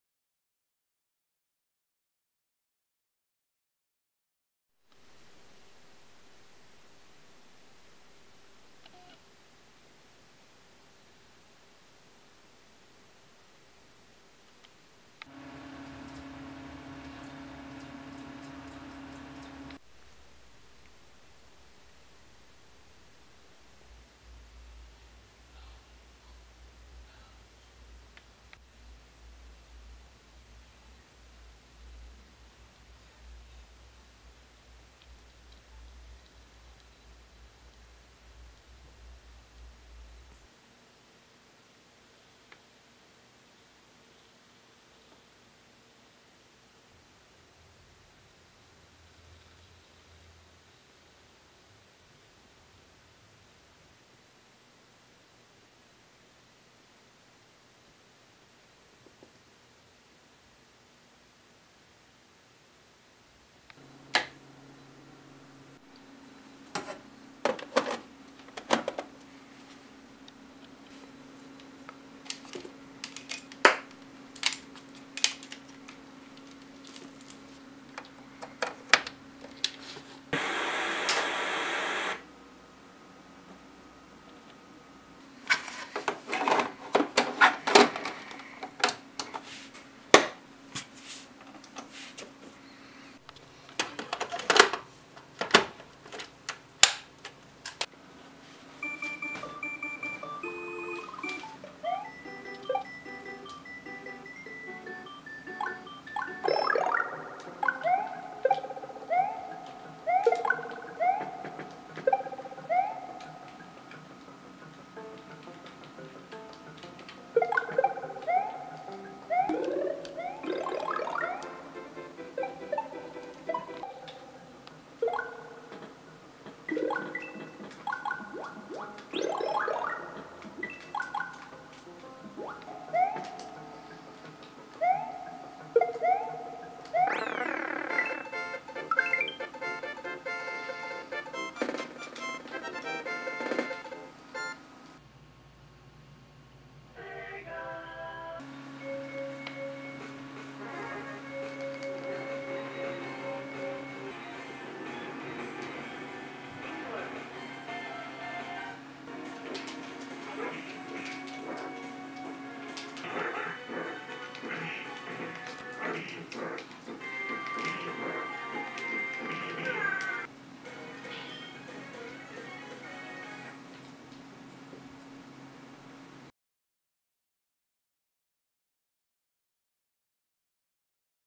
Immersion (It's more than a game)
For this project, I wanted to immerse the viewer in a very nostalgic atmosphere, specifically something many of us have experienced in our childhood - video games. I wanted to portray the games and accessories almost in a toy story fashion in the beginning with the lighting, almost as if they're waiting silently to played with. Then, I wanted to focus on the little sounds such as clicks on the power switch, the pushing of buttons, and the sounds effects coming from the game. The close up eye shots are meant to show how a video game can really take you to another world in the comfort of your own home, and how immersive a video game can truly be regardless of how old it is.